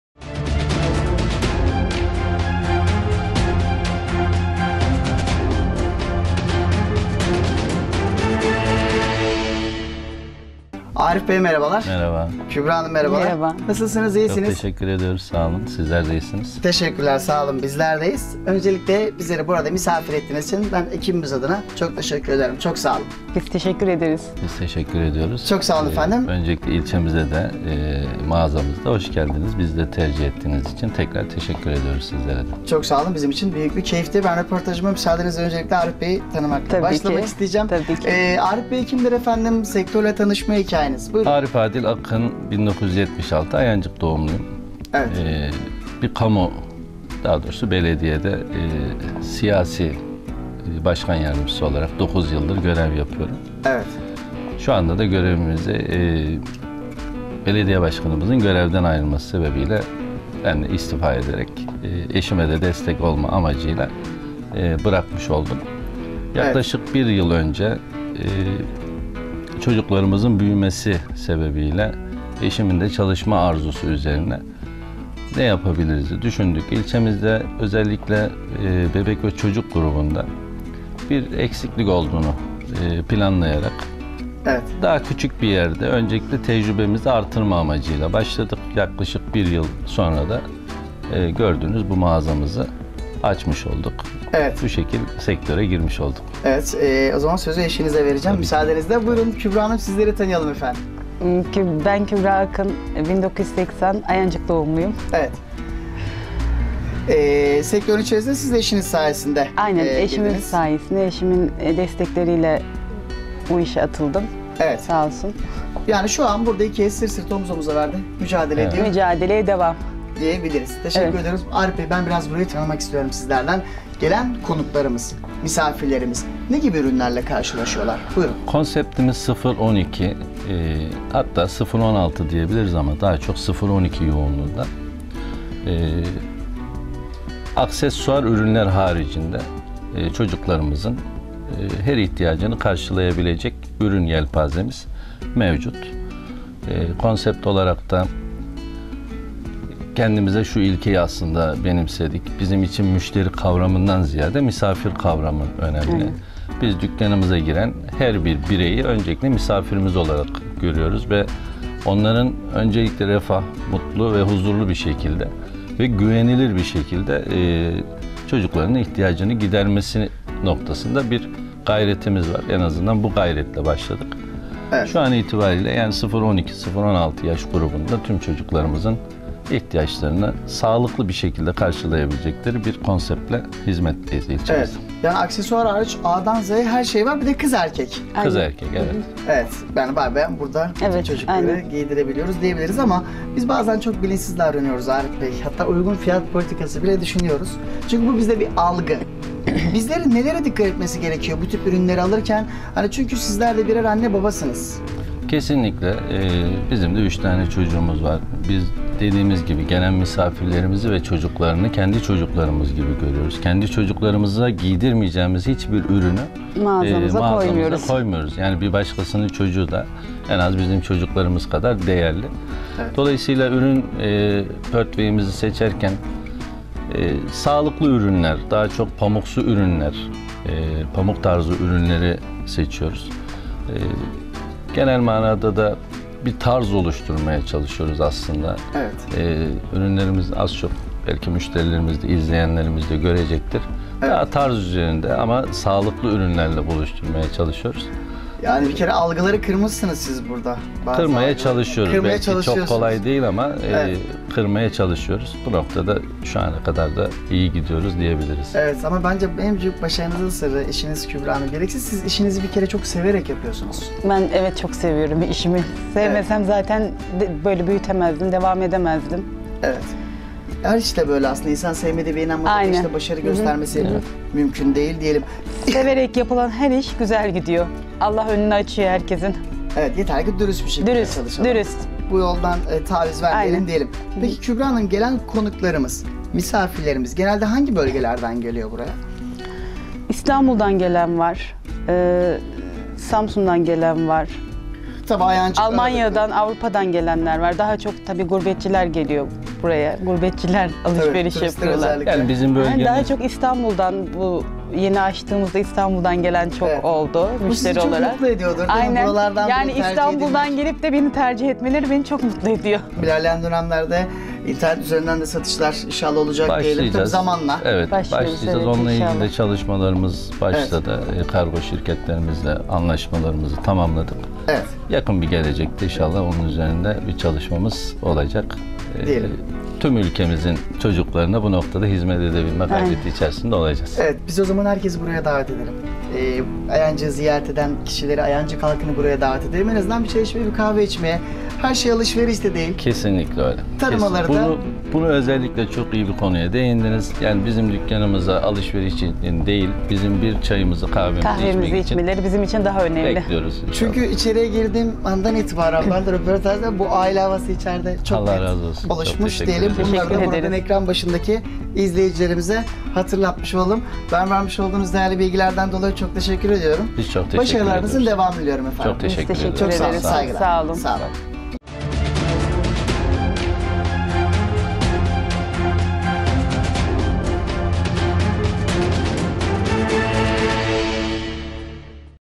KUBİŞ KIDS
Kübiş Kids firma sahibi Arif Adil Akın,  başarı ve faaliyetlerini bugün kobi özel'e anlattı.